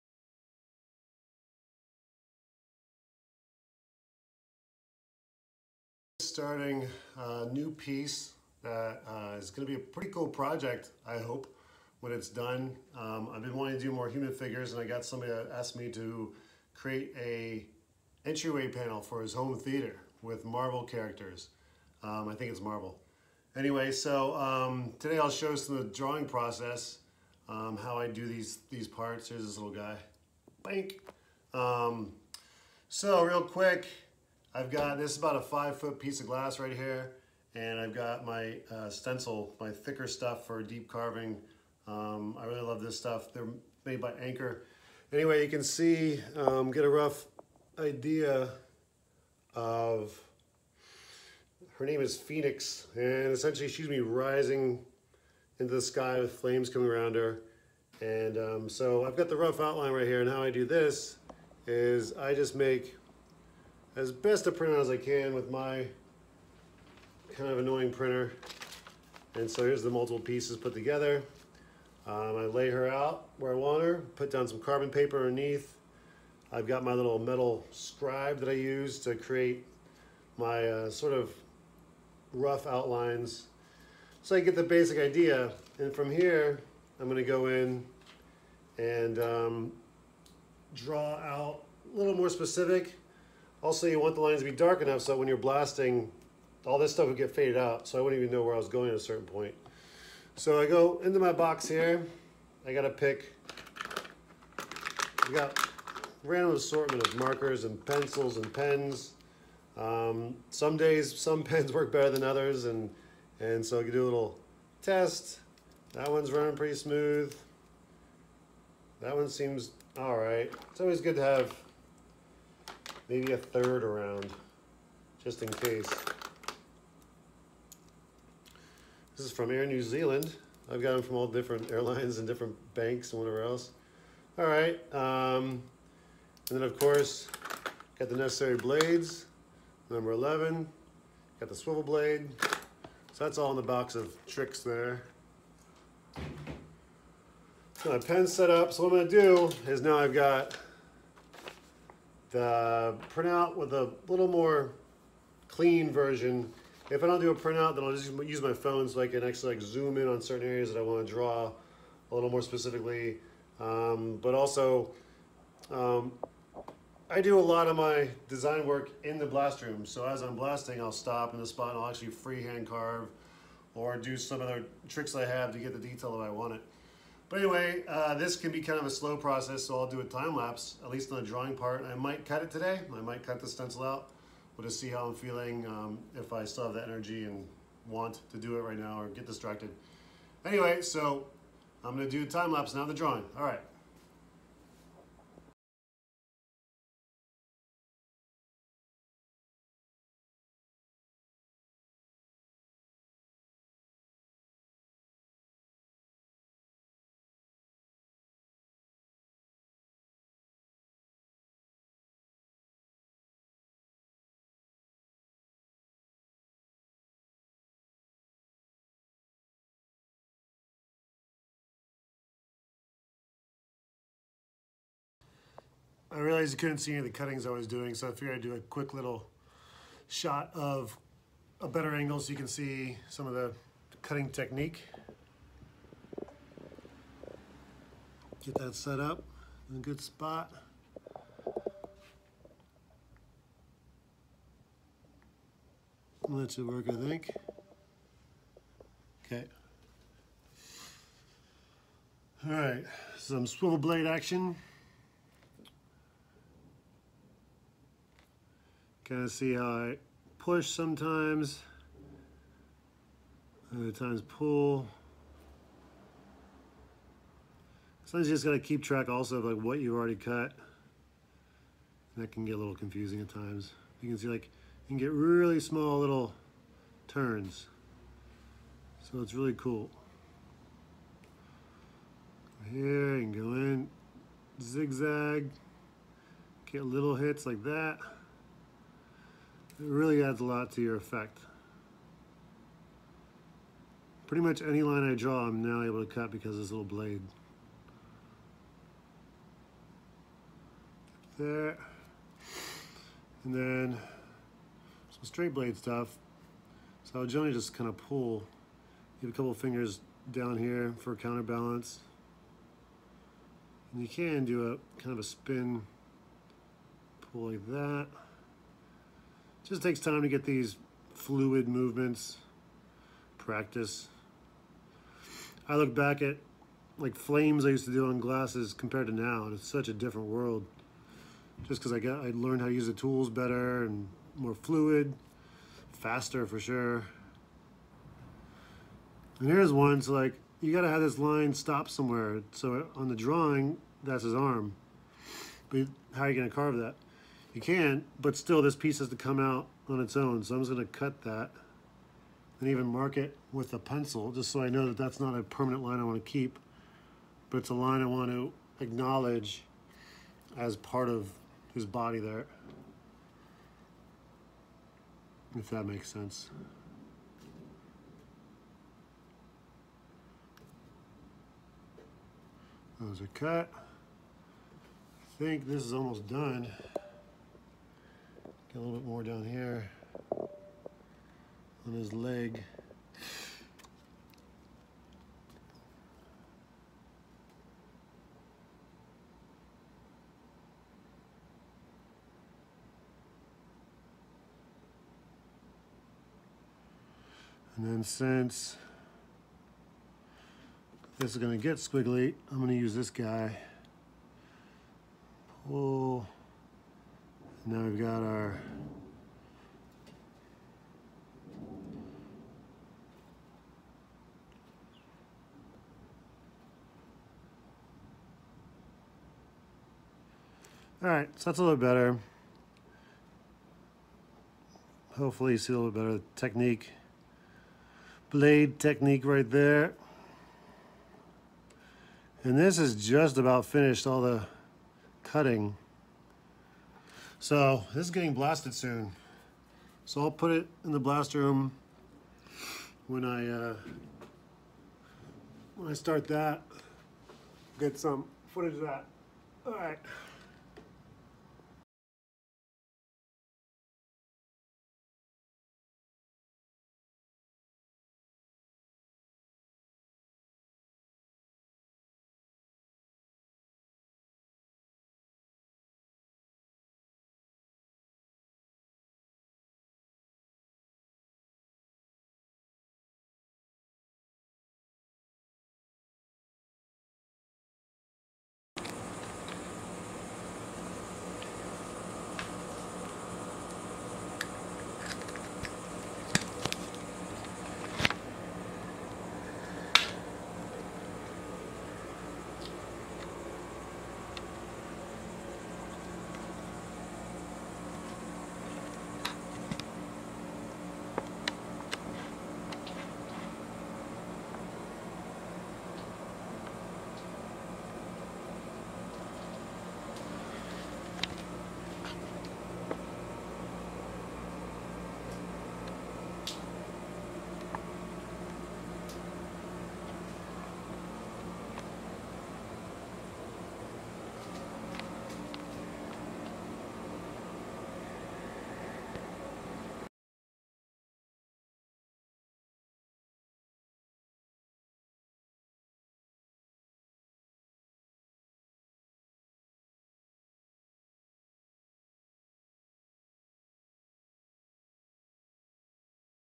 Goodwin Glassworks #7 Creating a stencil and blasting
Here are some pointers on how I create my designs in glass...hand drawn is the best way to create a style of your own. This particular piece features Marvel comic characters and shows the finished product